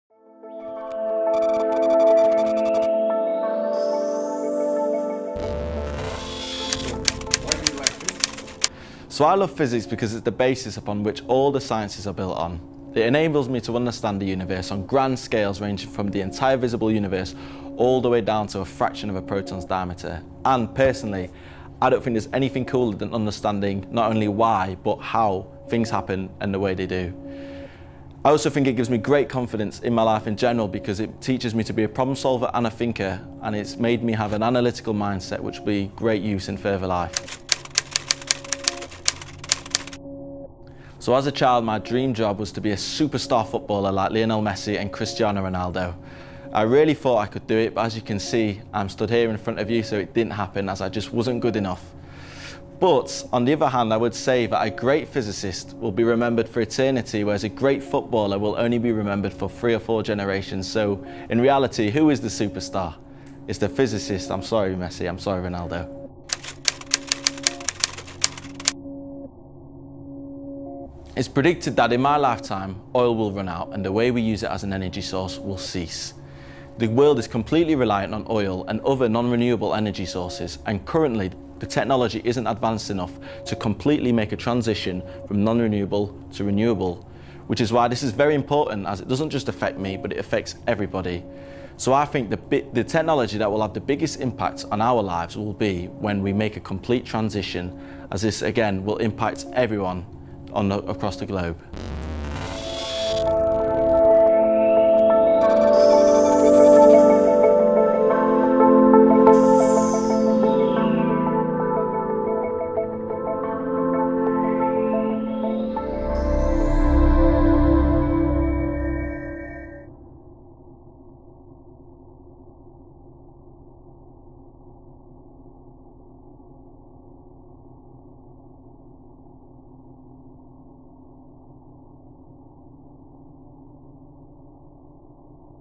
"I love Physics" with Kyle Dawson
Episode 7 of the series "I Love Physics"

British physics student Kyle Dawson talks about why he loves physics.

Copyright: CERN